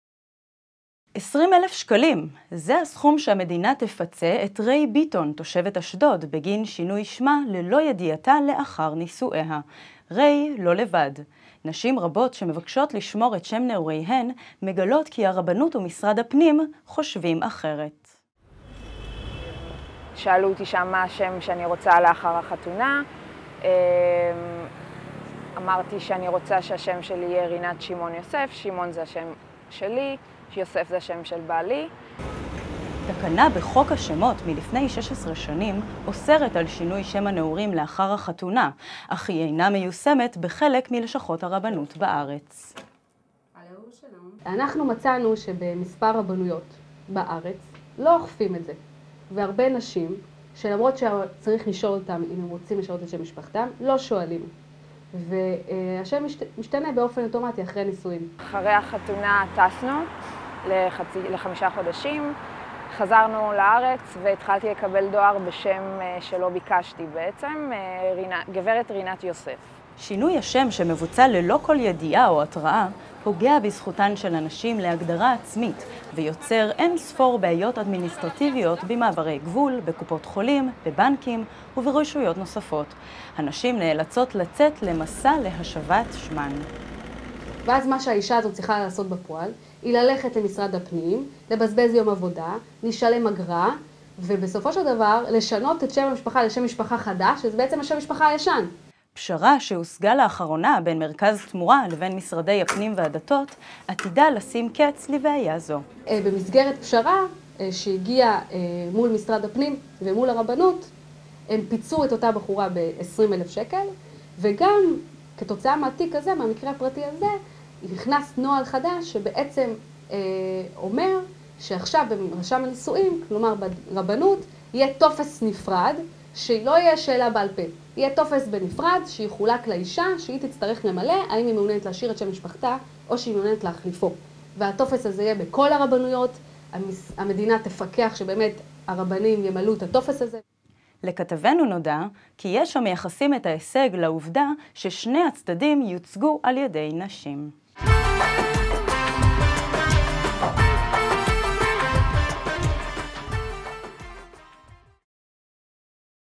נשים בוחרות את שמן
נשים רבות שמבקשות לשמור את שם נעוריהן, מגלות כי הרבנות ומשרד הפנים חושבים אחרת. אחת מהנשים היא ריי ביטון, תושבת אשדוד, שבגין שינוי שמה ללא ידיעתה לאחר נישואיה, תפוצה על ידי המדינה ב-20 אלף שקלים. שינוי השם שמבוצע ללא כל ידיעה או התראה, פוגע בזכותן של הנשים להגדרה עצמית ויוצר אין ספור בעיות אדמיניסטרטיביות במעברי גבול, בקופות-חולים, בבנקים וברשויות נוספות. הנשים נאלצות לצאת למסע להשבת שמן.